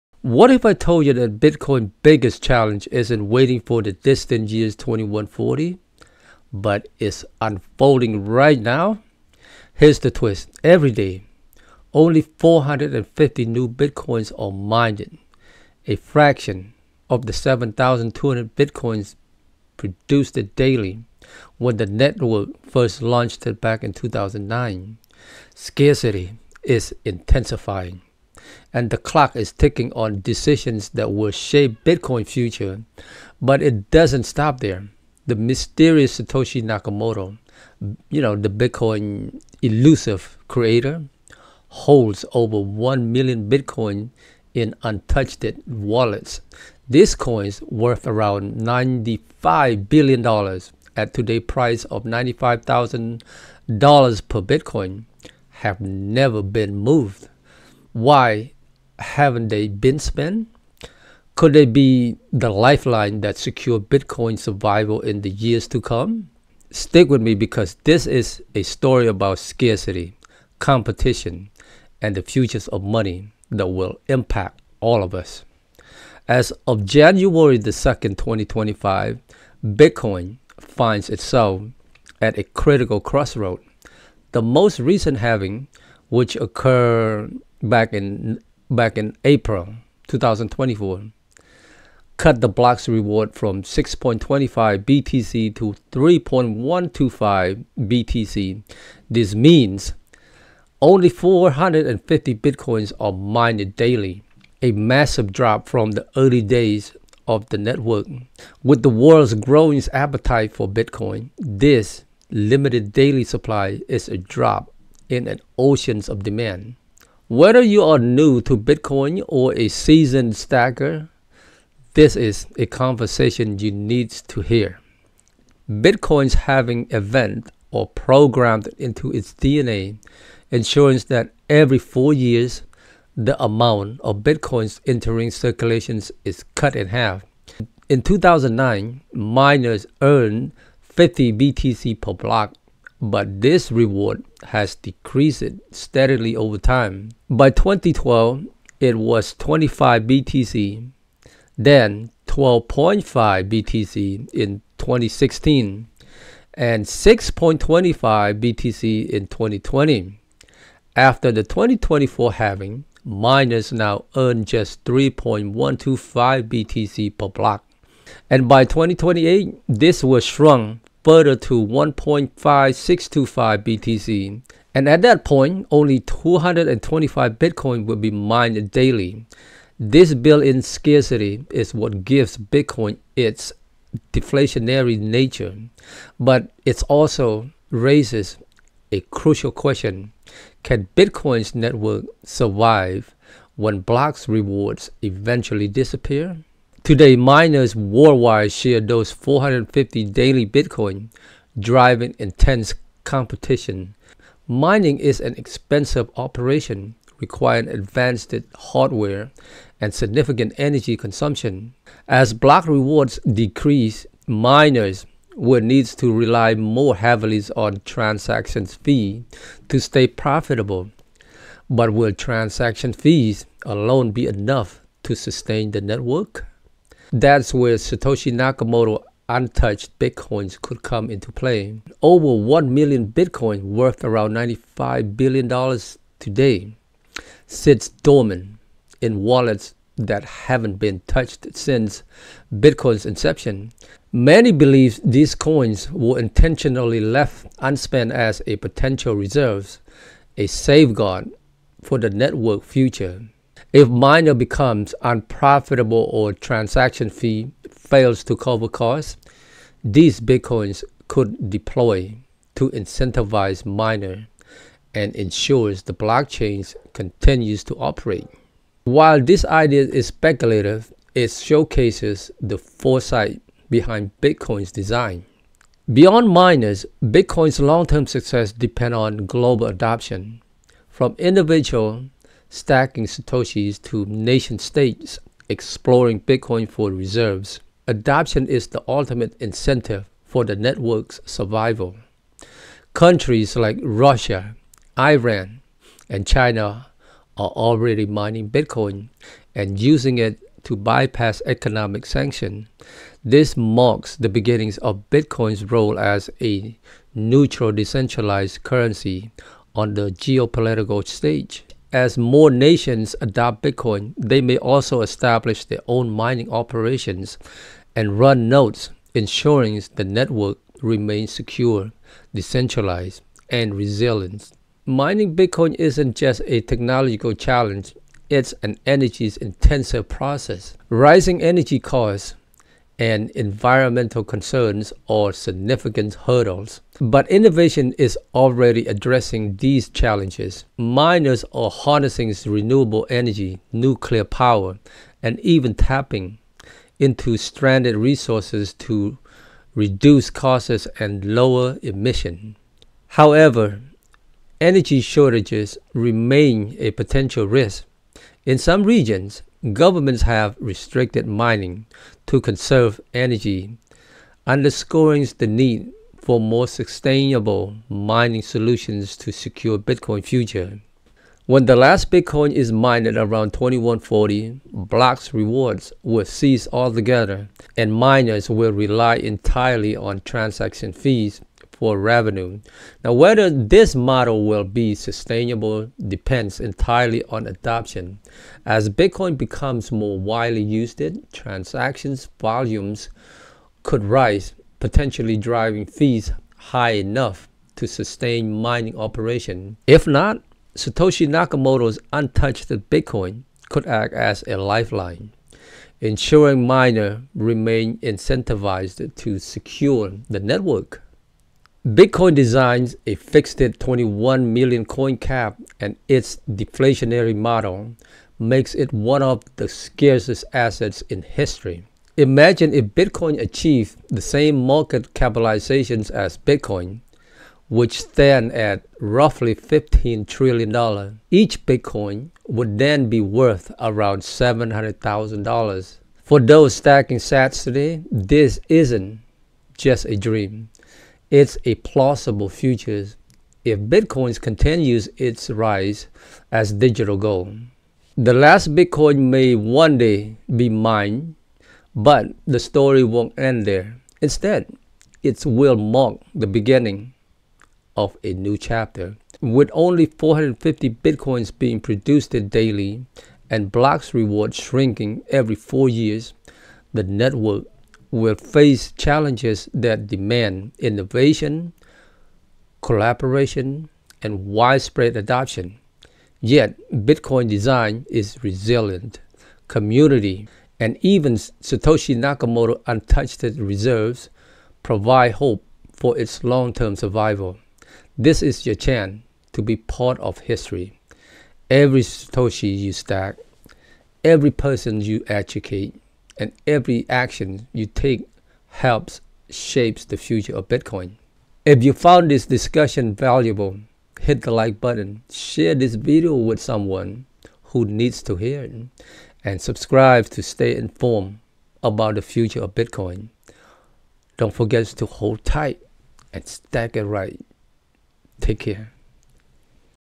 The Last Bitcoin Mined...What happens Next?
Description:

What happens when the last Bitcoin is mined? The clock is ticking as Bitcoin’s supply dwindles, with only 450 new coins entering circulation each day. Scarcity is intensifying, competition among miners is fiercer than ever, and the network’s long-term survival faces critical questions. Join us as we explore Bitcoin’s built-in scarcity model, the challenges miners face as block rewards decrease, and the fascinating role of Satoshi Nakamoto’s untouched $95 billion Bitcoin reserve. Could these dormant coins hold the key to Bitcoin’s future security?

In this video, we dive into the world of Bitcoin’s halving cycles, the impact of decreasing block rewards, and the global adoption shaping its destiny. From geopolitical shifts to energy challenges and the looming question of transaction fee sustainability, we’ll uncover the major forces influencing Bitcoin’s next chapter. Whether you’re a seasoned investor or just getting started, this discussion will deepen your understanding of the most transformative asset of our time. Don’t miss this compelling look at Bitcoin’s journey toward the 21 million cap—and what happens next.